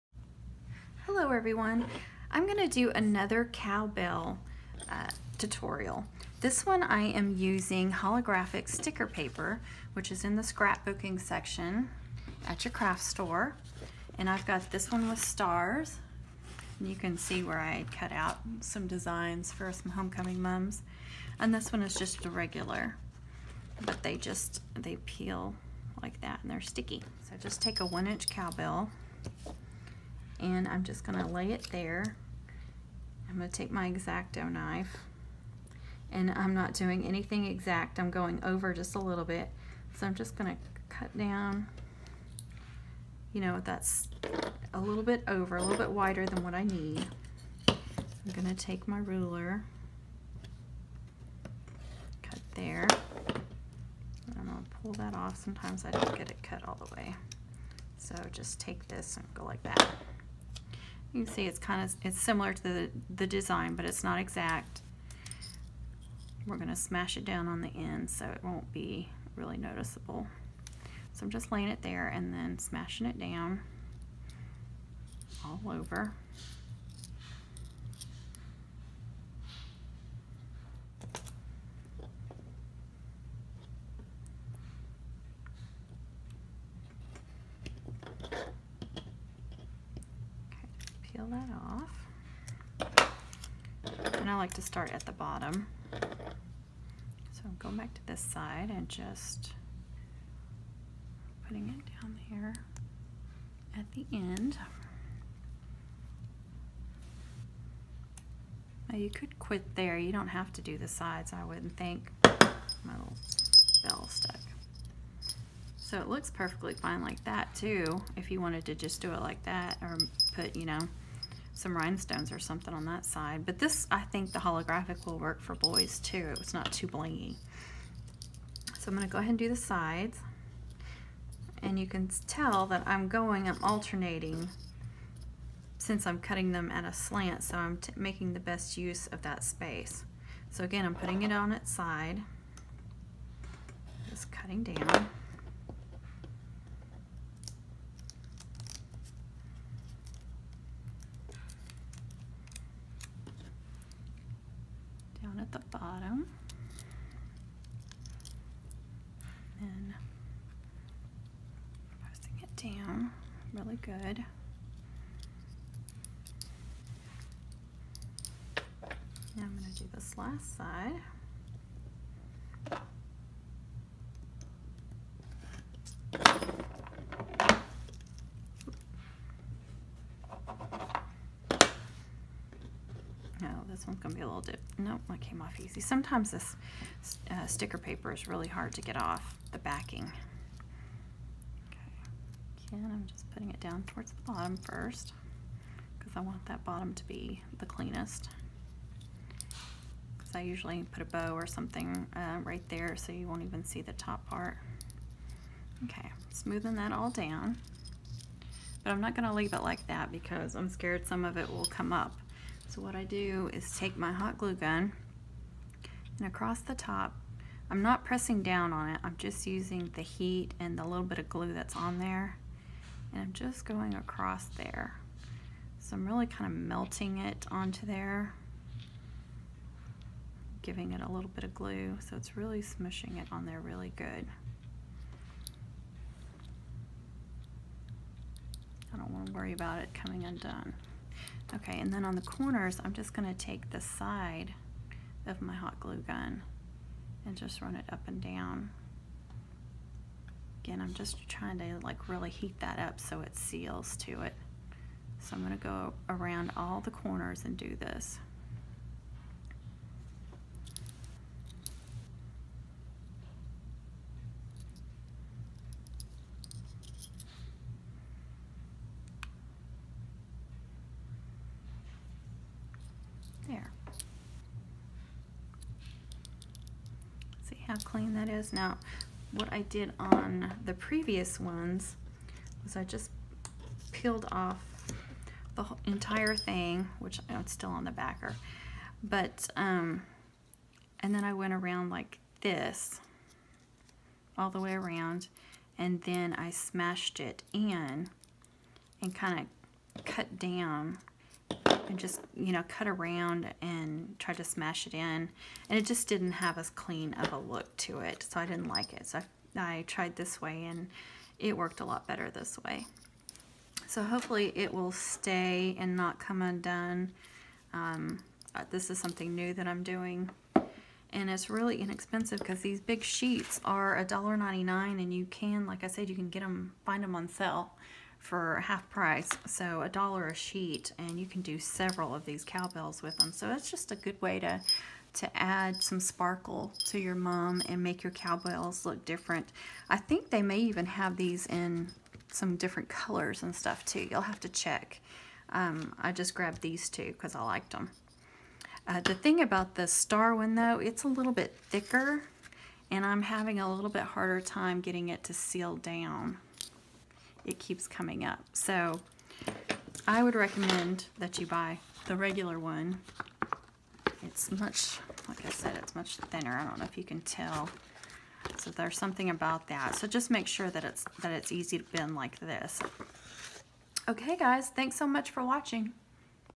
How To Make Holographic cow bells For Homecoming Mums; how to make homecoming mums; DIY MUM
1” Silver cow bells (hobby lobby)

Holographic sticker paper (Scrapbooking section @ craft stores).